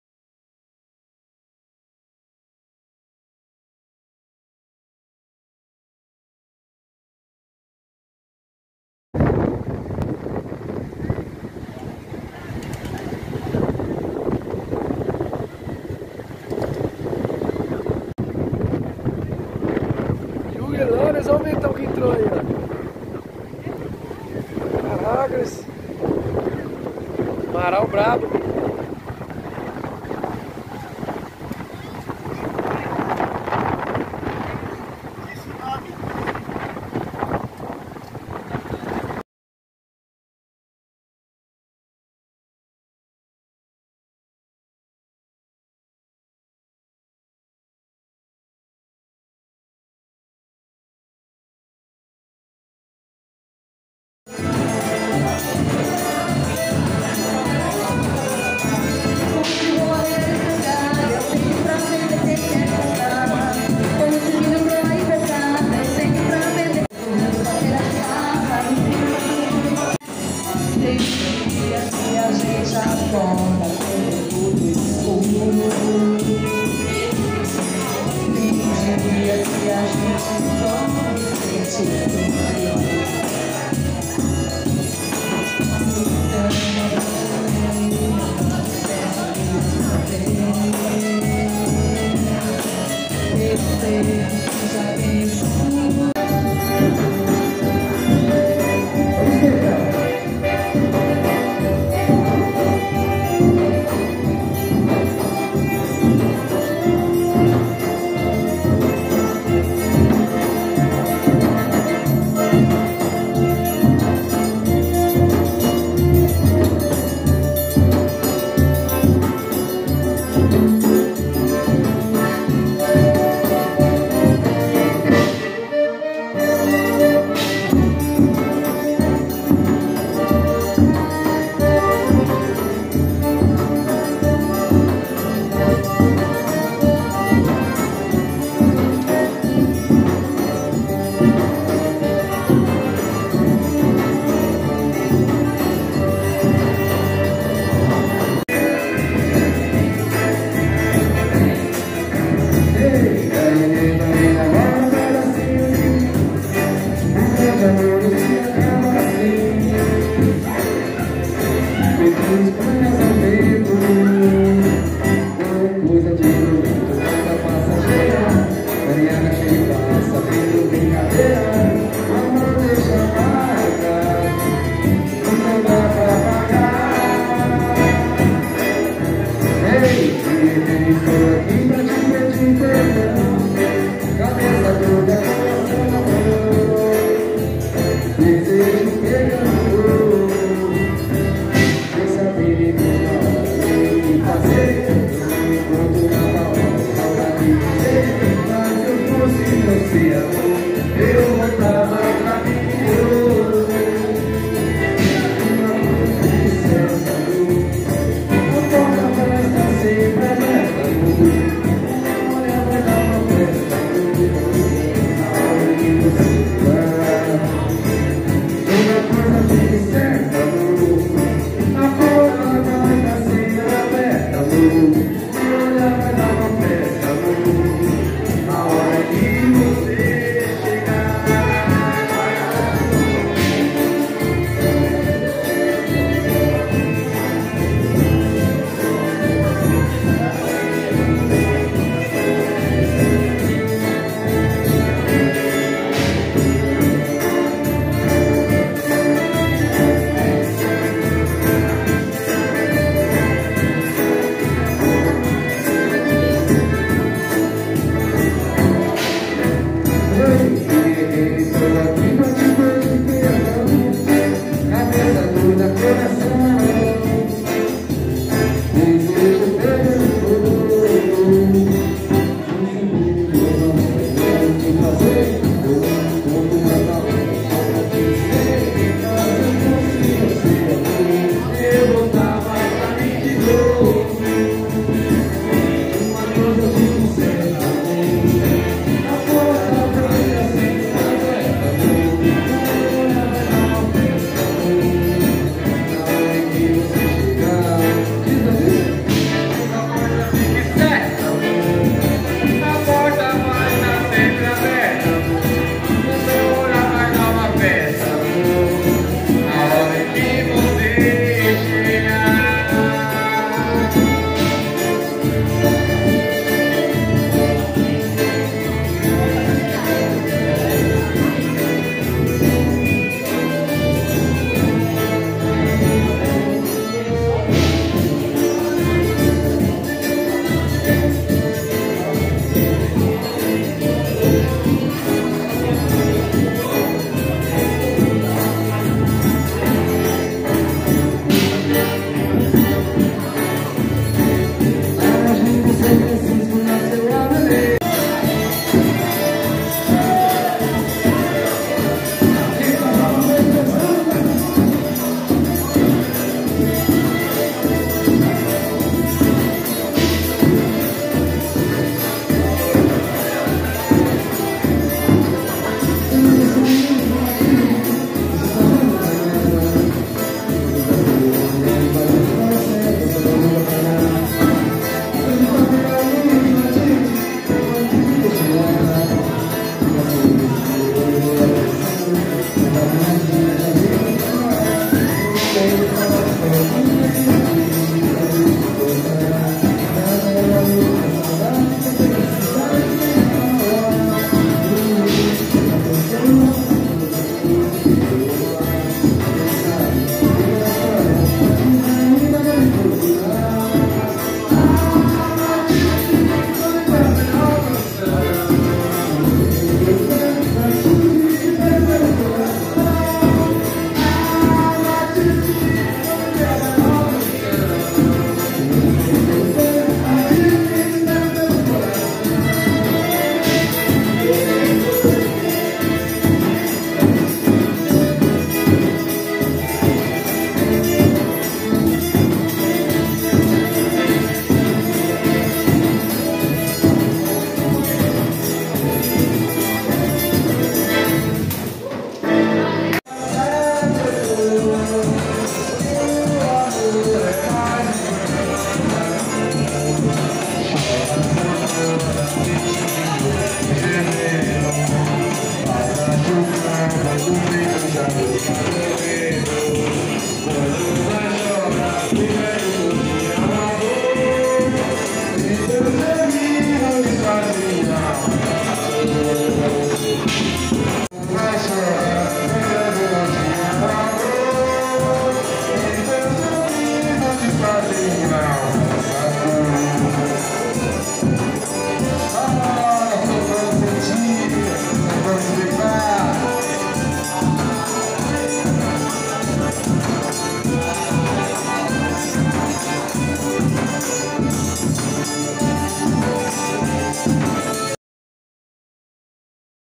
PE DE CHUMBO
uma apresentação do melhor grupo genuíno do Litoral Norte.